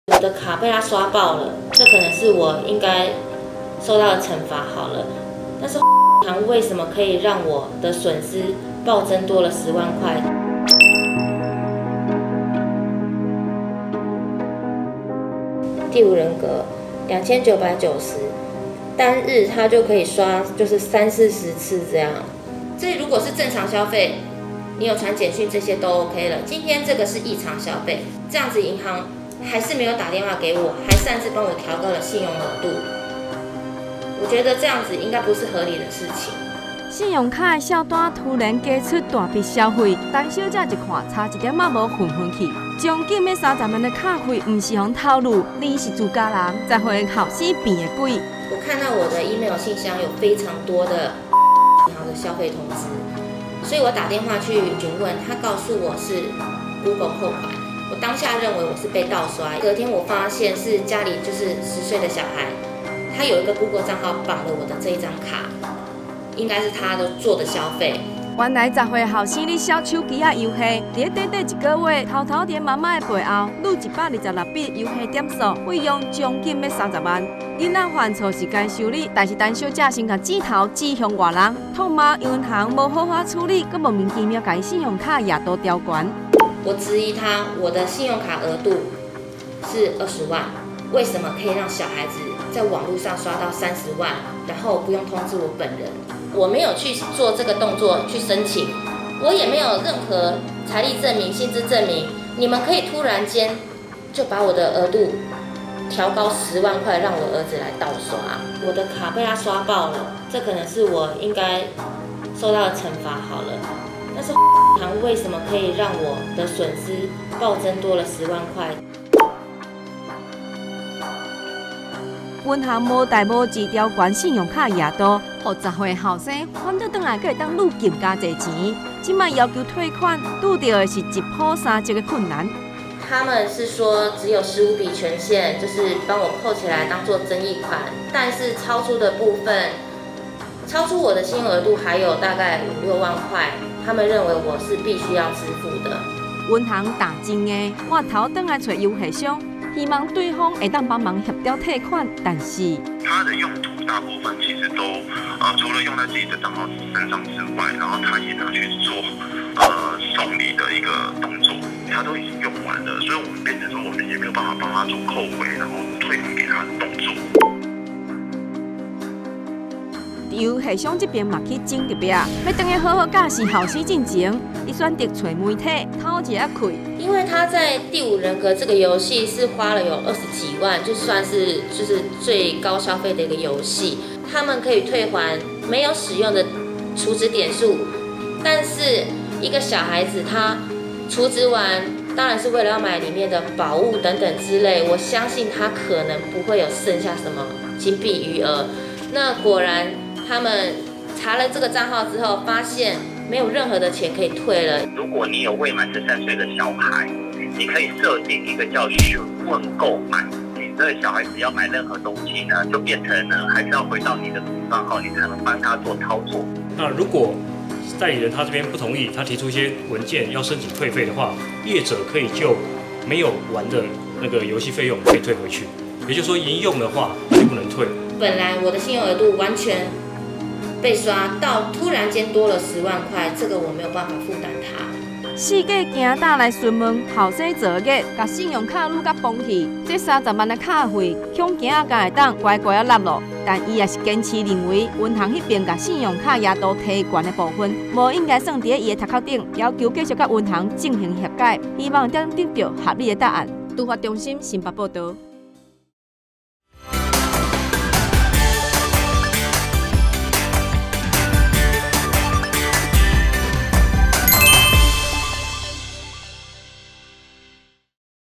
台語片 新北市一名陳姓辣媽收126封電郵悲劇了 10歲兒闖禍！瘋手遊月刷30萬 獨家 | 台灣新聞 Taiwan 蘋果新聞網
新北市一名陳姓媽媽，上月20日在電子信箱中突然發現數十封信用卡消費紀錄，檢查信用卡就在皮夾中，直覺是遭人盜刷立即申請爭議款項並停卡，事後拿出手機帳戶檢查才赫見，竟是10歲兒子偷刷信用卡爆買手遊點數，一個月就刷了126筆近30萬元的費用，她嘆「兒子打了也沒用」，趕緊與信用卡及遊戲商是否能協調退款。

今日熱門：【獨家】女嬰慘遭掐頸淒厲狂哭　竟是辣媽想挽回小王爛招